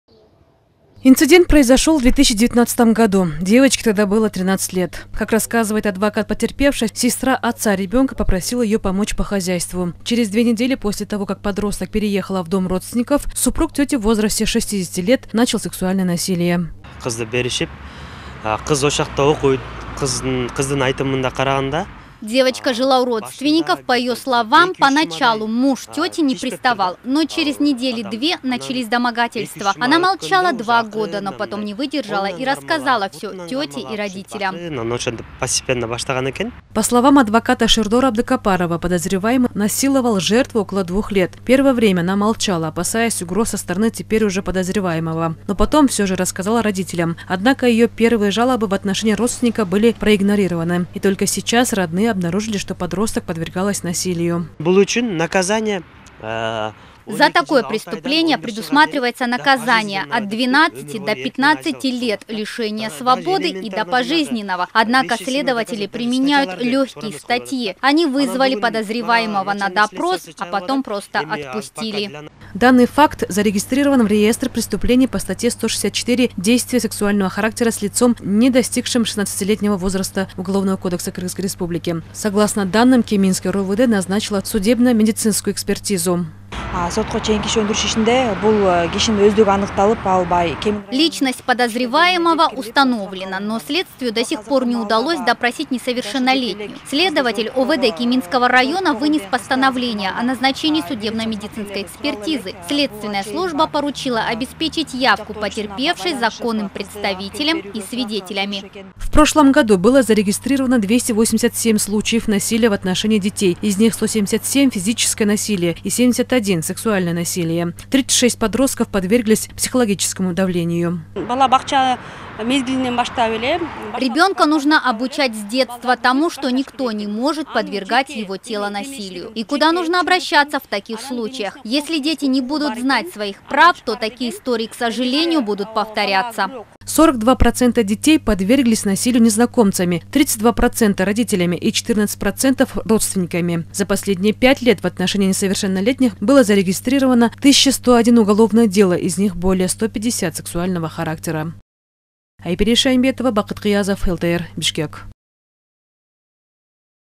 ИЗНАСИЛОВАНИЕ ДЕВОЧКИ: НАЧАЛОСЬ СЛЕДСТВИЕ
58-летний мужчина подозревается в изнасиловании 13-летней девочки. Инцидент произошел в 2019 году. Как рассказывает адвокат потерпевшей, сестра отца ребенка попросила её помочь по хозяйству. Через две недели после того, как подросток переехала в дом родственников, супруг тети начал сексуальное насилие. По словам адвоката Шердора Абдыкапарова, первое время подросток молчала, опасаясь угроз. Но потом все же рассказала родителям. Данный факт зарегистрирован в ЕРПП по статье “Действия сексуального характера с лицом, не достигшим шестнадцатилетнего возраста” УК КР.